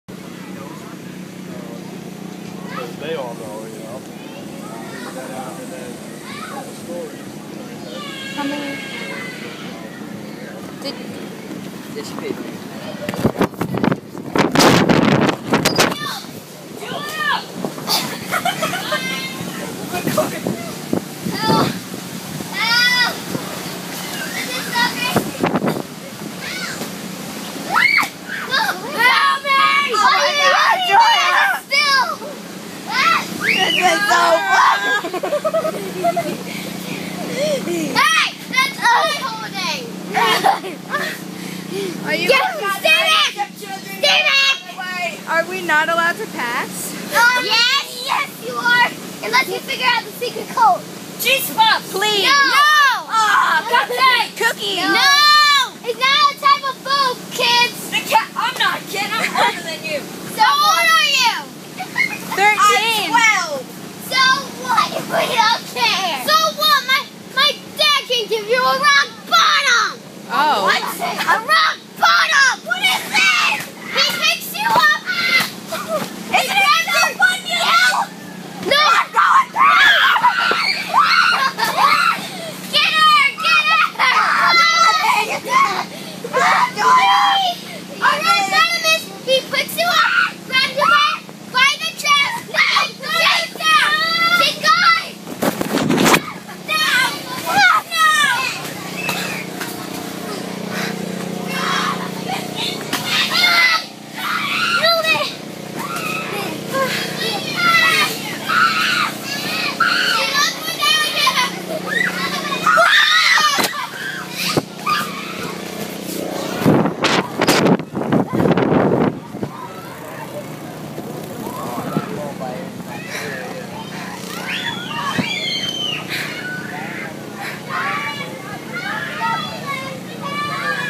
We were attacked and chased by little kids
They were really mean